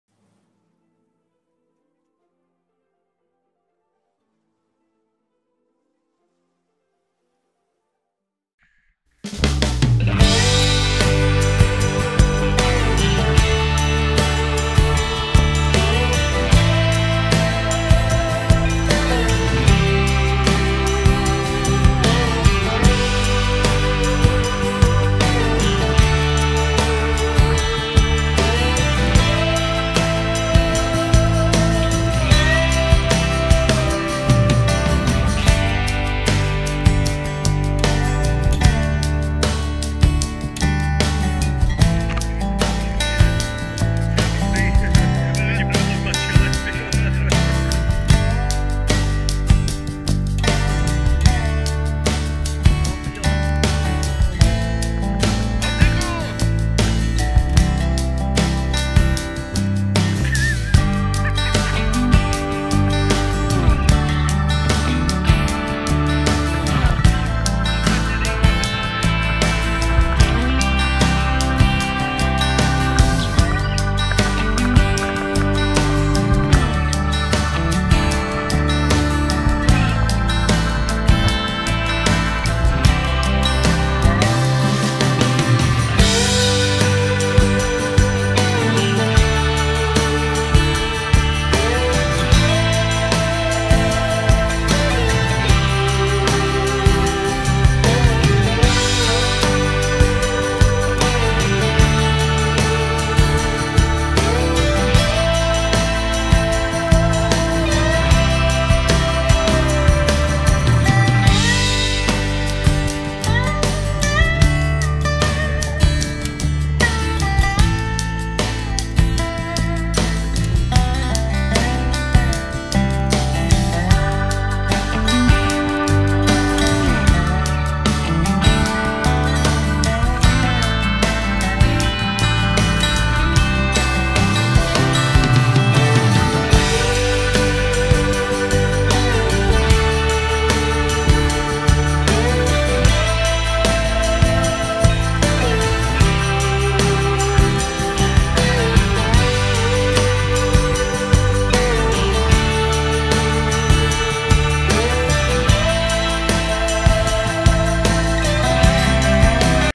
Potápění - Bořená hora 13.1.2019
První ponor pod ledem.
Děkujeme Honhovi za vypůjčení kamery, Kubovi za support a Lence za  ponor.